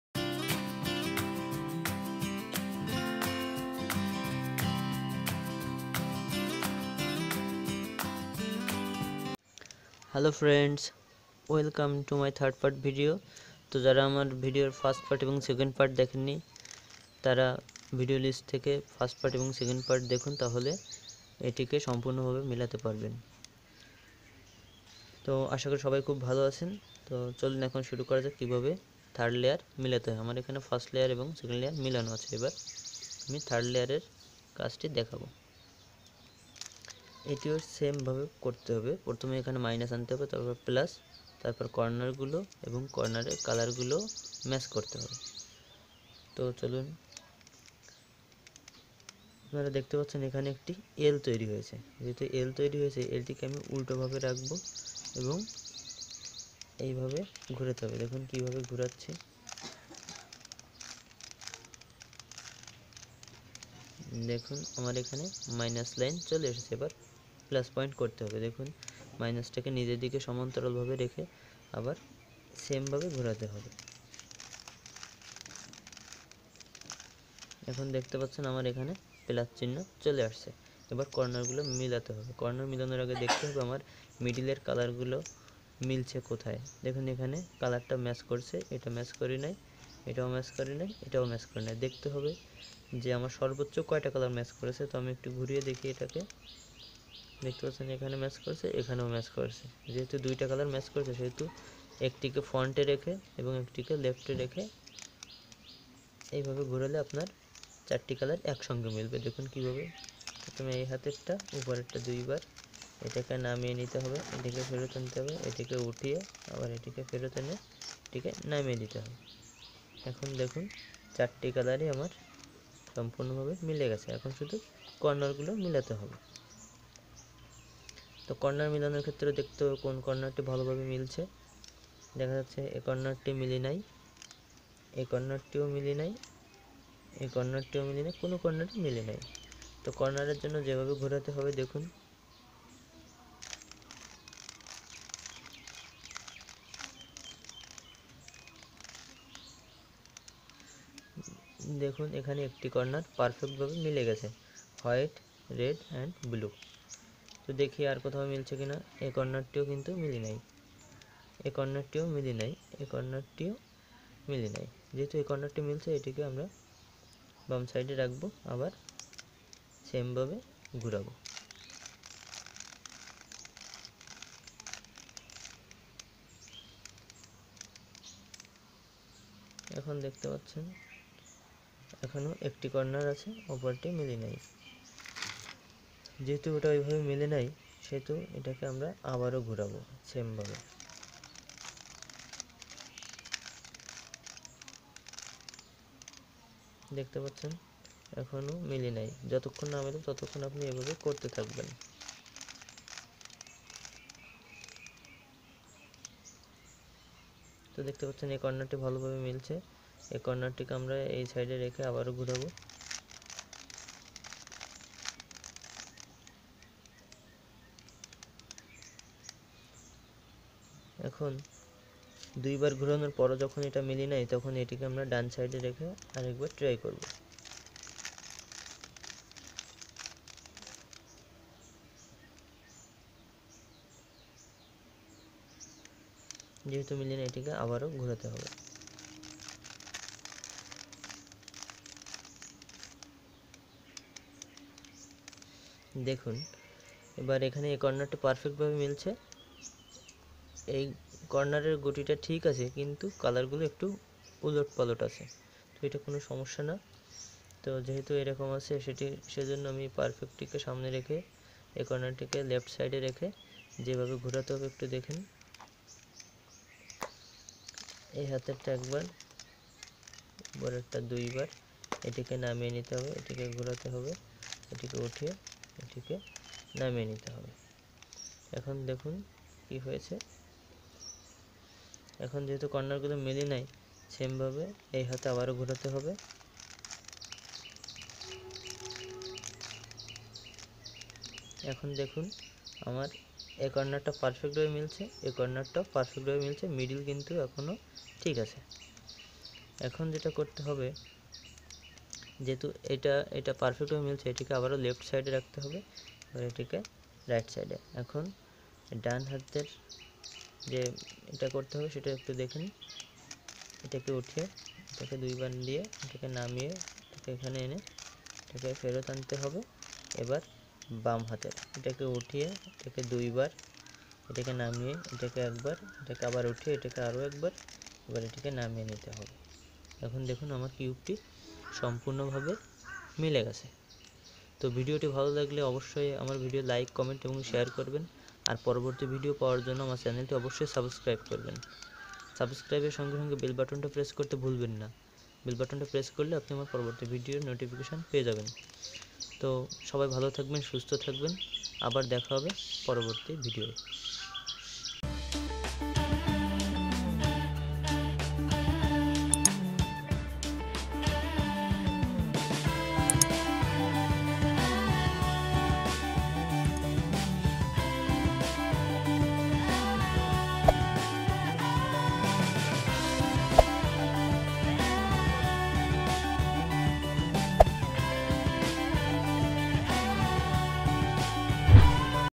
How To Solve 3*3 Rubik's Cube. সহজ পদ্ধতিতে কিউব মিলানো. 3*3 Rubik's Cube Part - 3
Hi friends,
i am Anjan Ray . Today, i am show to you How to Solve 3*3 Rubik's Cube. সহজ পদ্ধতিতে কিউব মিলানো . 3*3 Rubik's Cube Part -3